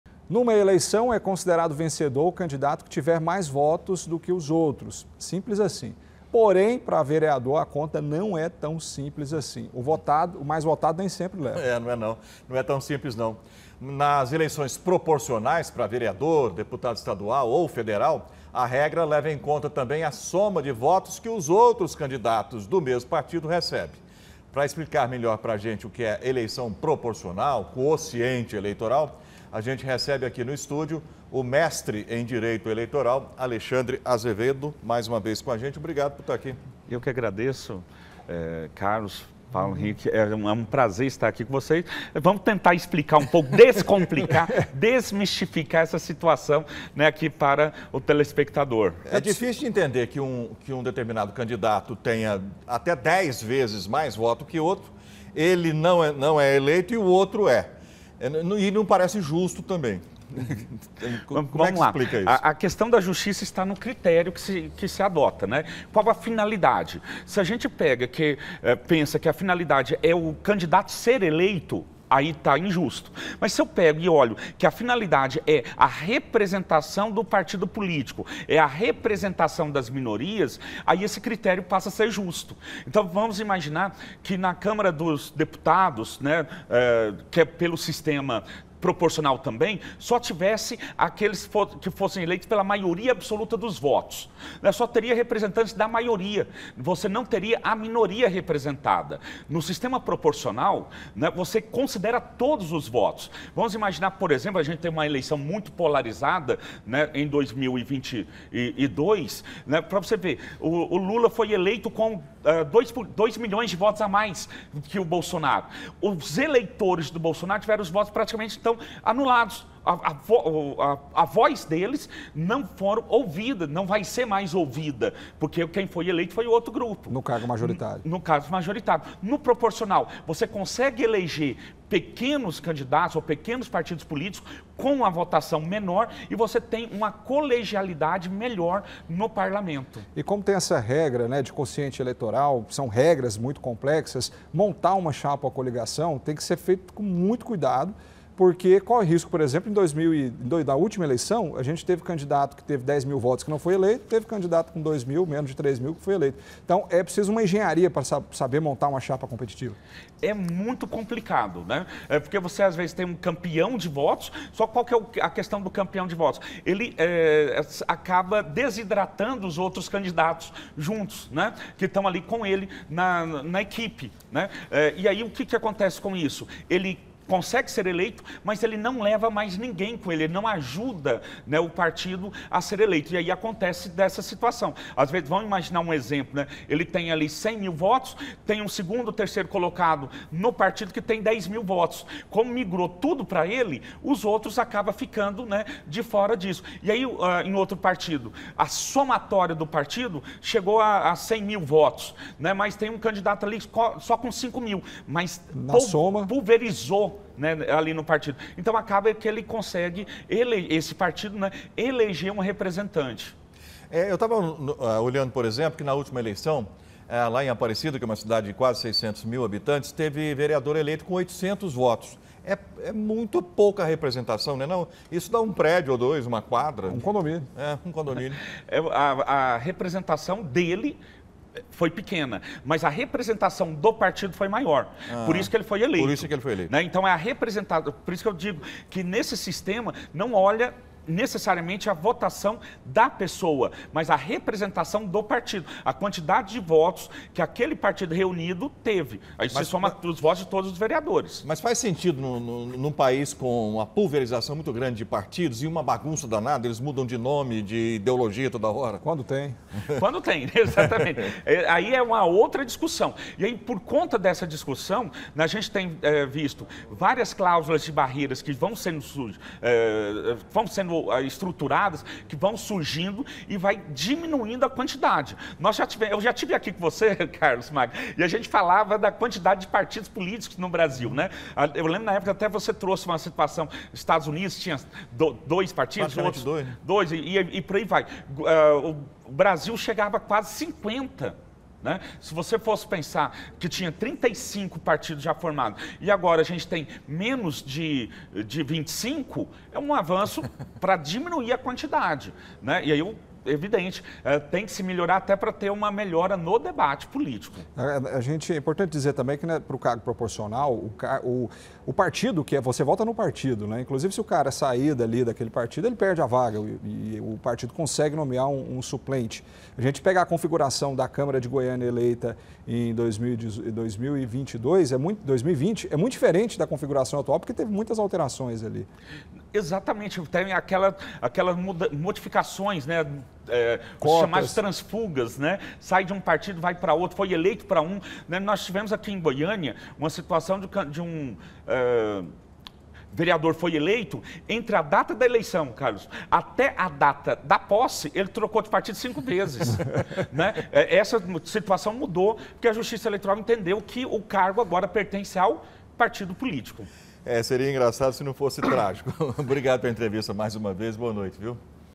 POLÍTICA EM DEBATE: ENTENDA COMO SERÁ A ELEIÇÃO DE VEREADORES
NUMA ELEIÇÃO, É CONSIDERADO VENCEDOR O CANDIDATO QUE TIVER MAIS VOTOS DO QUE OS OUTROS. SIMPLES ASSIM. PORÉM, PRA VEREADOR A CONTA NÃO É TÃO SIMPLESINHA NÃO. O VOTADO NEM SEMPRE LEVA. NÃO É TÃO SIMPLES MESMO NÃO. NAS ELEIÇÕES PROPORCIONAIS, PRA VEREADOR, DEPUTADO ESTADUAL OU FEDERAL, A REGRA LEVA EM CONTA TAMBÉM A SOMA DE VOTOS  QUE OS OUTROS CANDIDATOS DO PARTIDO RECEBE. PRA EXPLICAR MELHOR PRA GENTE O QUE É ELEIÇÃO PROPORCIONAL, QUOCIENTE ELEITORAL, A GENTE RECEBE AQUI NO ESTÚDIO, O MESTRE EM DIREITO ELEITORAL , ALEXANDRE
AZEVEDO.